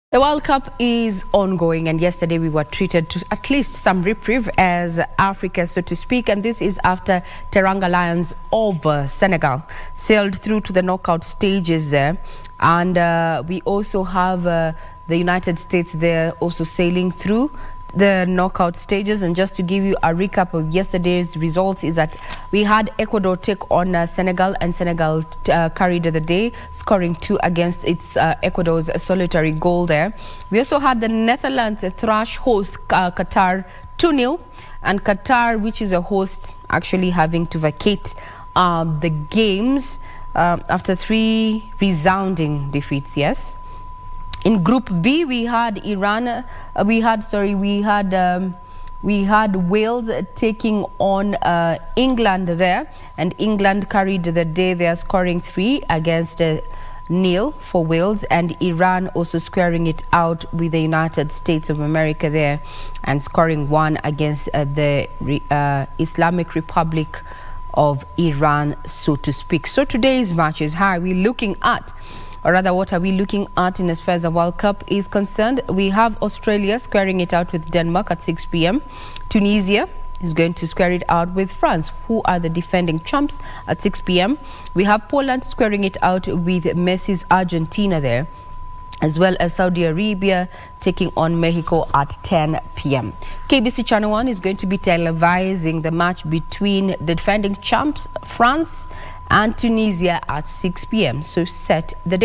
Bruno Fernandes scores a brace to send Portugal through a knockout stage after beating Uruguay 2-0.
Bruno Fernandes scored a brace to send Portugal through to the knockout stages from Group H with a game to spare after beating Uruguay 2-0.